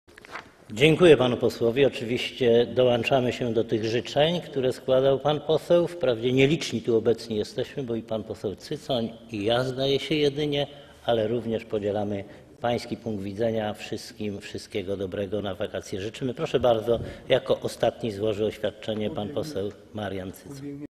[260/262] Eugeniusz Tomasz Grzeszczak: Dziękuję panu posłowi. Oczywiście dołączamy się do ...
Posiedzenie Sejmu nr 46
26 lipca 2013 r. 15:24:48
Wicemarszałek Eugeniusz Tomasz Grzeszczak:

Oczywiście dołączamy się do życzeń, które składał pan poseł. Wprawdzie obecni tu nie są liczni, bo, zdaje się, jedynie pan poseł Cycoń i ja, ale podzielamy pański punkt widzenia. Życzymy wszystkim wszystkiego dobrego na wakacje.

Proszę bardzo, jako ostatni oświadczenie wygłosi pan poseł Marian Cycoń.